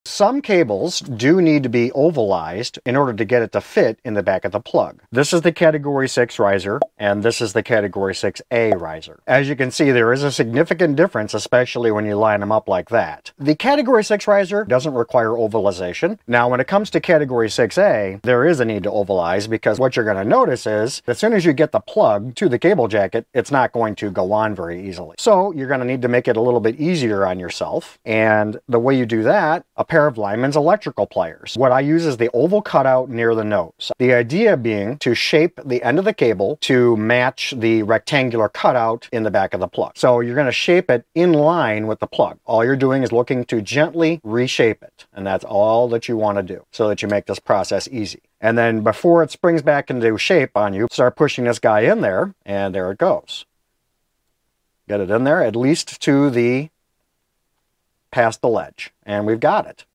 Making Your Cable Fit Your Connector
"Thick Ethernet” is not some sort of new type of Ethernet cable. What we mean by “thick” is literally that...thick. Sometimes you need to make it thinner to get it properly set up, and that is where this video is here to help!

For more information on this subject, check out our step-by-step blog